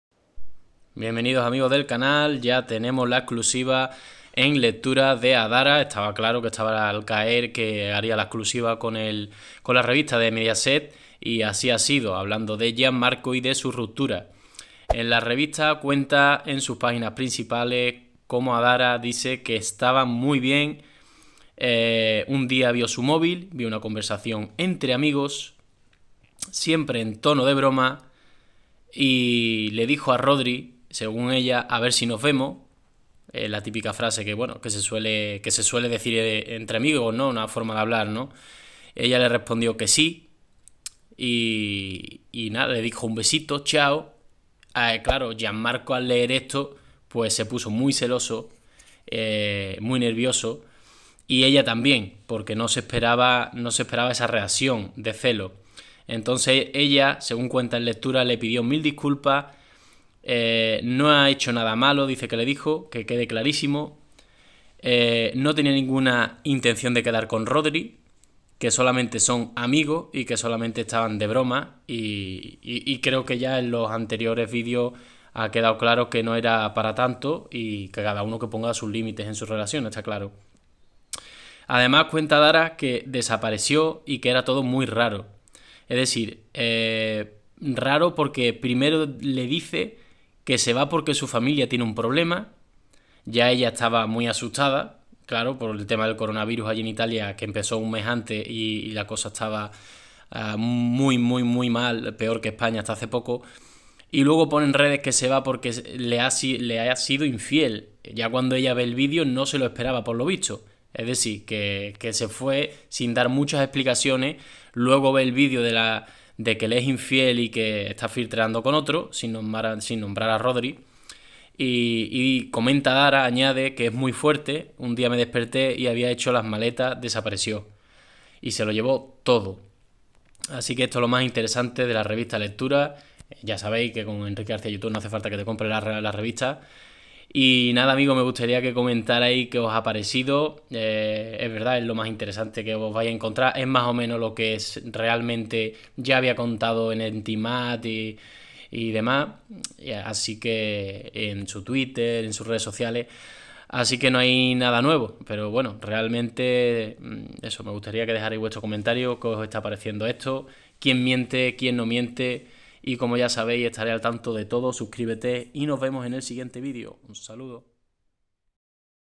Adara EXCLUSIVA LECTURAS " Gianmarco es MALO"
Exclusiva para Lecturas de Adara. Habla de que Gianmarco le mintió y es malo.

GOT TALENT, GH VIP, SUPERVIVIENTES, LA ISLA, EL HORMIGUERO

Gracias